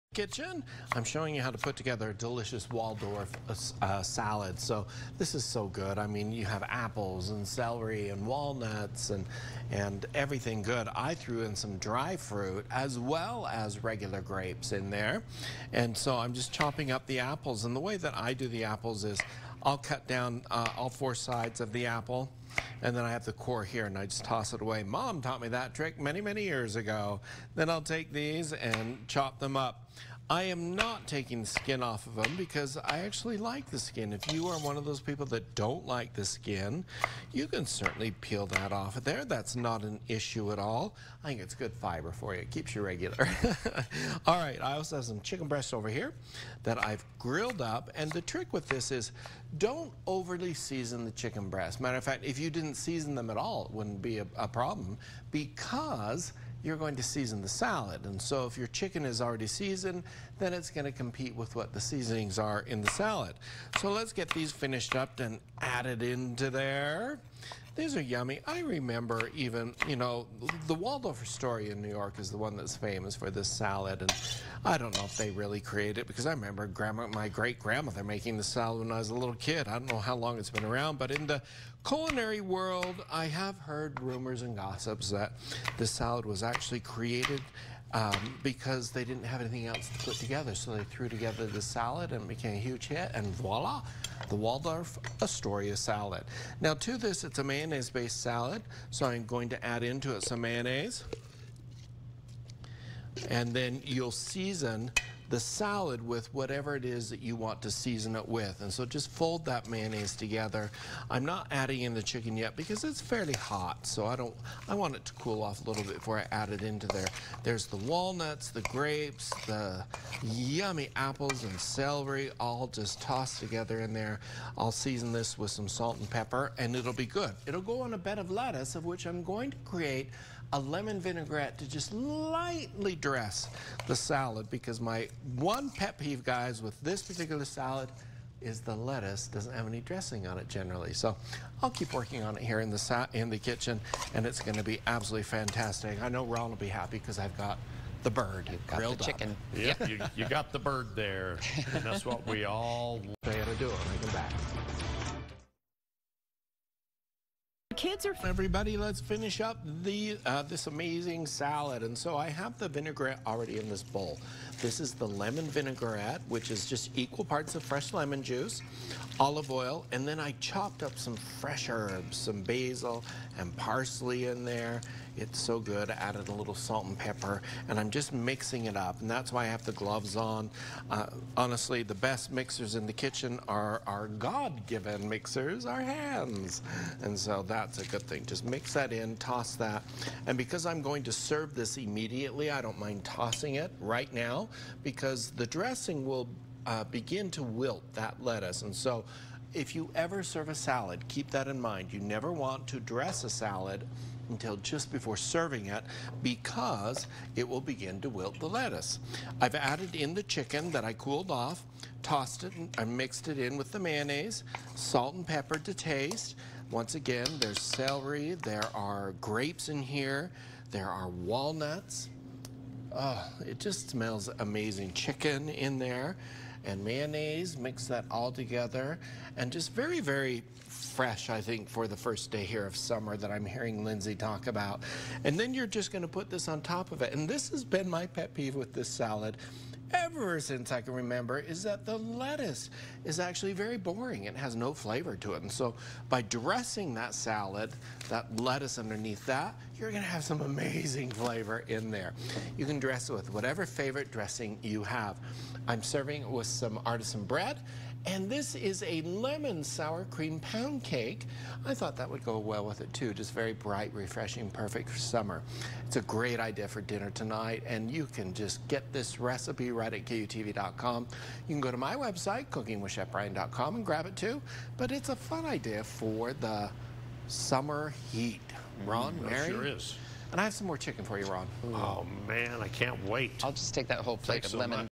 Cooking with Chef Bryan: Grilled Chicken Waldorf Salad with Lemon Herb Dressing
Treat yourself to a delightful twist on a classic dish with our Grilled Chicken Waldorf Salad. This refreshing recipe combines tender grilled chicken with crisp apples, crunchy celery, and sweet grapes, all topped with a zesty lemon herb dressing. Perfect for a healthy lunch or light dinner, this salad is both delicious and nutritious.

Threads: @kutv2news